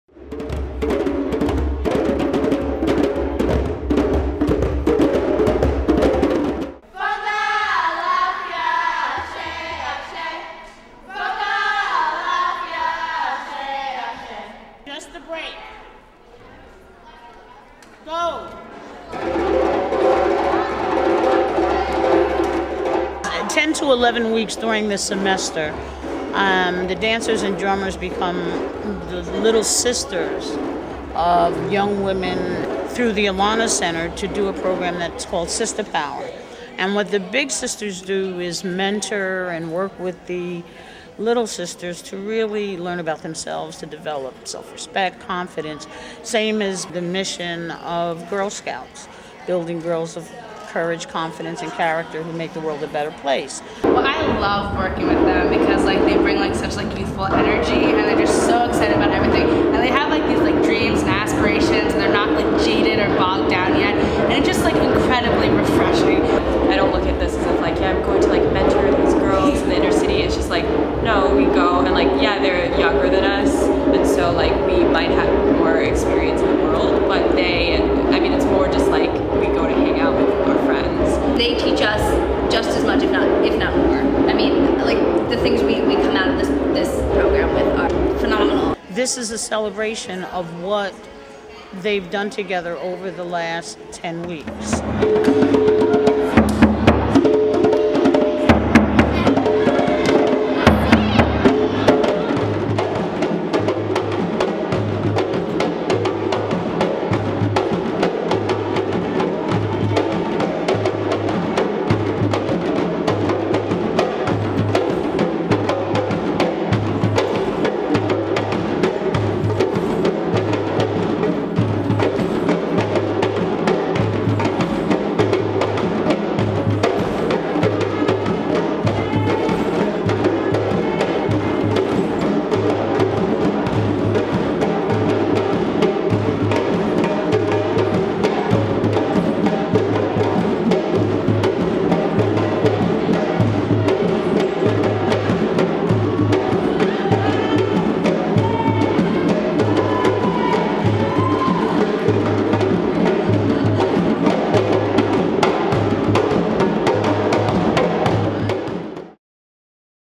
We Play Drums
The year-end celebration of Sistah Power!--a collaboration between Vassar College and Dutchess County Girl Scouts